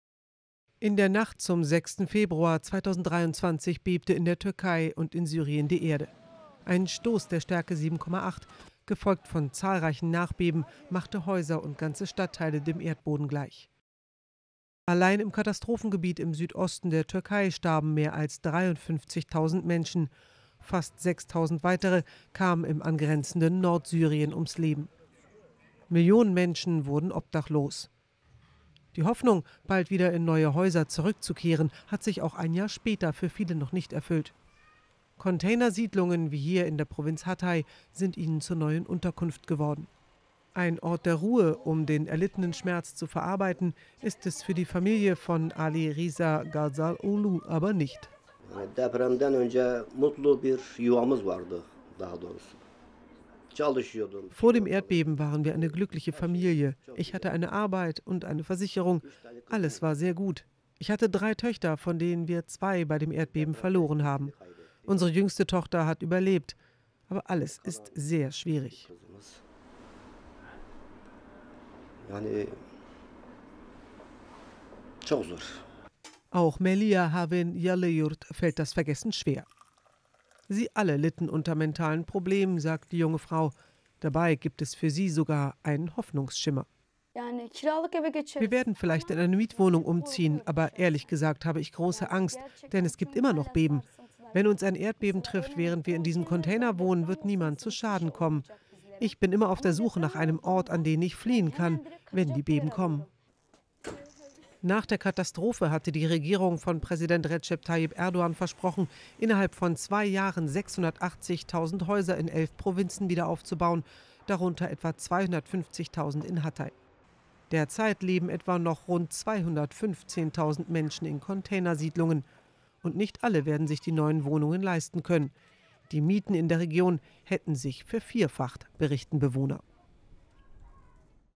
Ein Jahr nach dem Erdbeben: Überlebende kämpfen um Rückkehr in normales Leben
Vor einem Jahr kamen bei der Erdbebenkatastrophe in der Türkei und Syrien 58.000 Menschen ums Leben, Millionen wurden obdachlos. Heute leben allein in der türkischen Provinz Hatay noch mehr als 215.000 Menschen in Containersiedlungen.